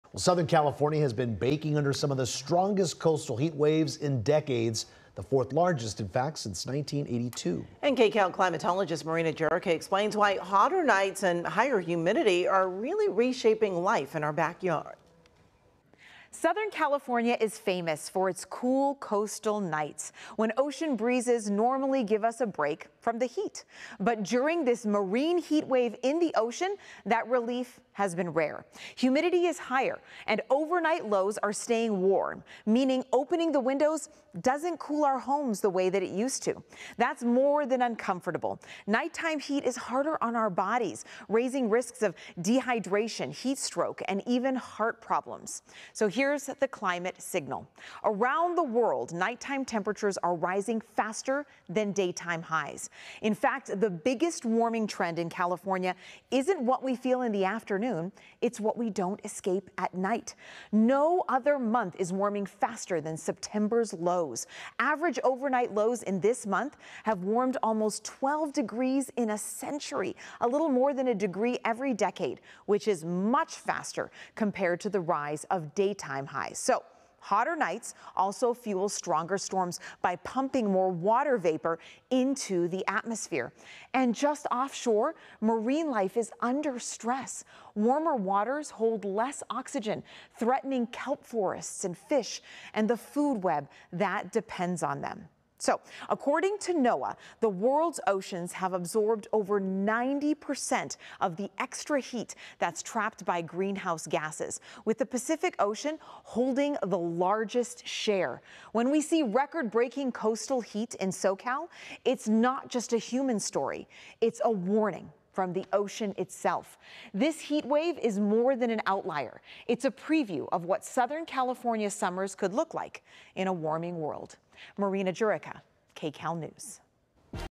Southern California swelters under some of strongest coastal heat waves in decades
KCAL News Climatologist Marina Jurica takes a look at the science behind a recent trend of coastal heat waves that are the strongest Southern California has seen in decades.